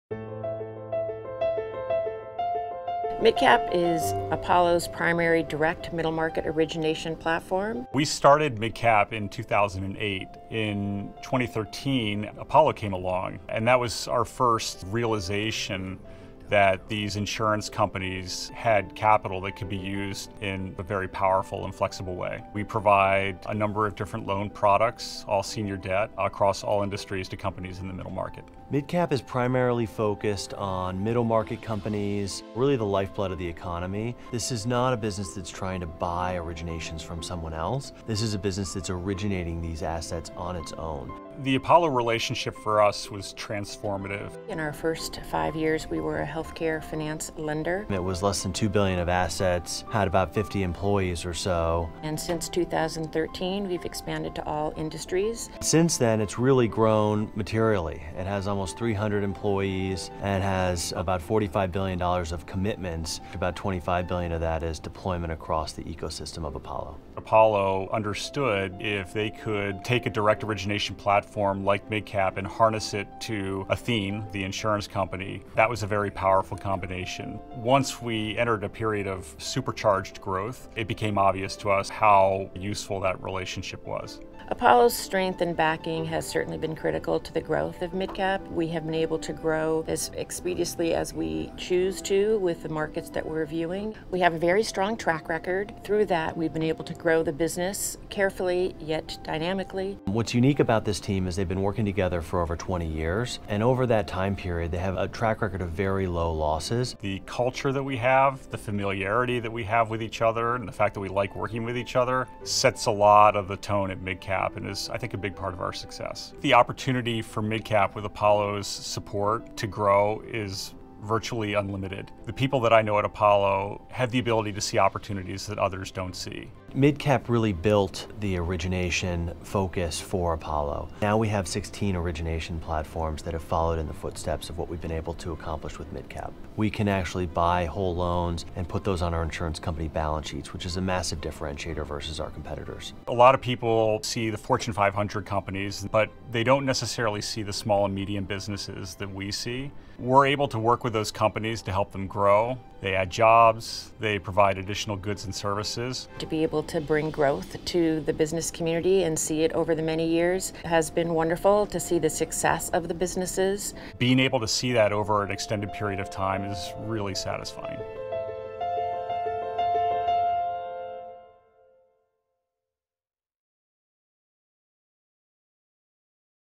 MidCap Financial I Apollo
Learn how MidCap Financial helped build the foundation of Apollo’s origination ecosystem, which now spans 16 specialty finance platforms.